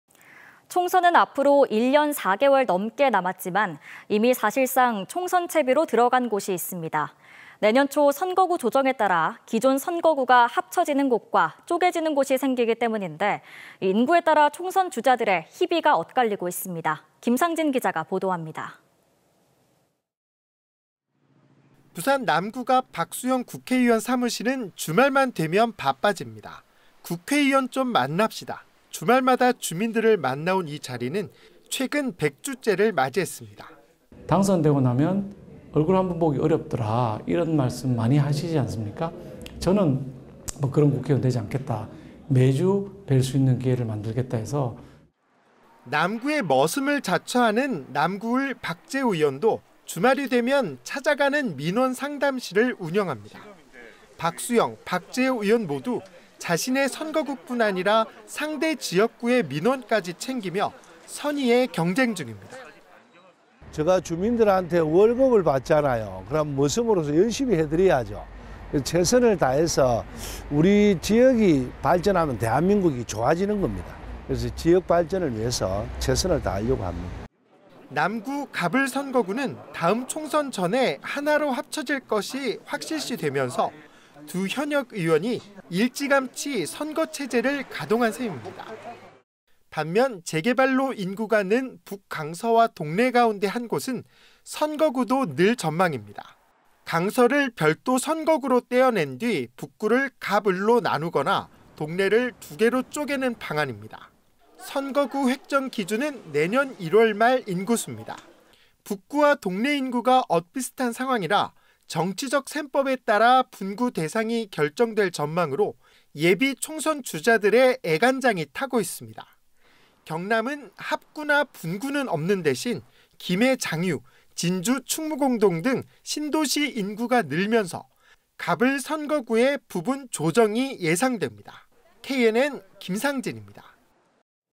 "인구수 따라 선거구 줄까, 늘까..."총선 주자 애간장
[앵커]
총선은 앞으로 1년 4개월 넘게 남았지만 이미 사실상 총선채비로 들어간 곳이 있습니다.

내년 초 선거구 조정에 따라 기존 선거구가 합쳐지는 곳과 쪼개지는 곳이 생기기 때문인데, 인구에 따라 총선주자들의 희비가 엇갈리고 있습니다.

김상진 기자가 보도합니다.

[기자]

부산 남구갑 박수영 국회의원 사무실은 주말만 되면 바빠집니다.

'국회의원 쫌 만납시다', 주말마다
주민들을 만나온 이 자리는 최근 100주째를 맞이했습니다.

{박수영/국회의원(부산 남구갑)"당선되고나면 국회의원 얼굴 한번 보기 어렵더라, 이런 말씀을 많이 하시지않습니까? 저는 그런 국회의원 되지않겠다, 매주 볼 수 있는 기회를 만들겠다해서..."}

남구 머슴을 자처하는 남구을 박재호 의원도 주말이 되면 찾아가는 민원상담실을 운영합니다.

박수영, 박재호 의원 모두 자신의 선거구 뿐 아니라 상대 지역구의 민원까지 챙기며 선의의 경쟁중입니다.

{박재호/국회의원(부산 남구을)"제가 주민분들께 월급을 받잖아요. 그런 머슴으로서 열심히 해드려야죠. 최선을 다해서 우리 지역이 발전하면 대한민국이 좋아지는 겁니다. 그래서 지역발전을 위해 최선을 다하려고 합니다"}

남구 갑*을 선거구는 다음 총선 전에 하나로 합쳐질 것이 확실시되면서 두 현역 의원이 일찌감치 선거체제를 가동한 셈입니다.

반면 재개발로 인구가 는 북*강서와 동래 가운데 한 곳은 선거구도 늘 전망입니다.

강서를 별도 선거구로 떼어낸 뒤 북구를 갑*을로 나누거나, 동래를 두개로 쪼개는 방안입니다.

선거구 획정 기준은 내년 1월
인구수입니다.

북구*동래 인구가 엇비슷한 상황이라 정치적 셈법에 따라 분구대상이 결정될 전망으로 예비 총선주자들의 애간장이 타고 있습니다.

경남은 합구나 분구는 없는 대신,

김해 장유, 진주 충무공동 등 신도시 인구가 늘면서 갑*을 선거구 부분 조정이 예상됩니다.

KNN 김상진입니다.